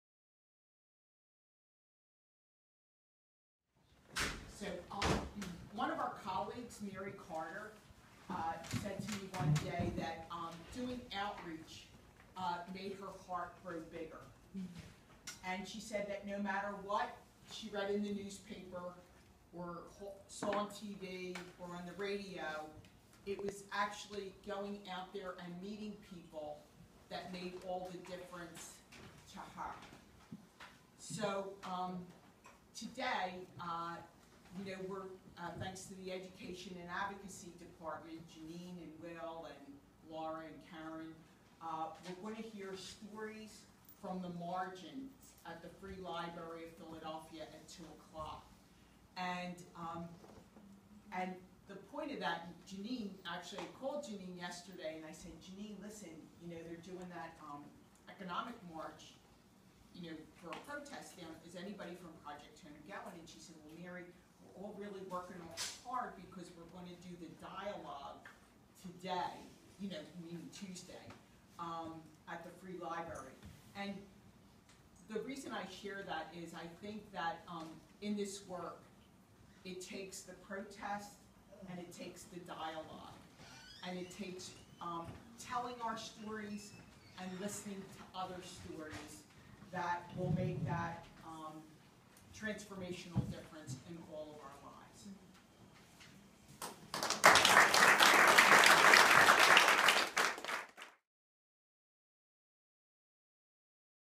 Morning Inspiration Meeting | July 26, 2016
Every Tuesday morning, the Project HOME community will be gathering for short, 20 minute meetings each week, that aim to gather us as a community, inspire us to start our day, ground us in our mission and values, and connect us to stories of struggle, recovery, transformation, and hope. We've decided to share portions of these gatherings with everyone in the hopes that you can draw some inspiration from them, as well!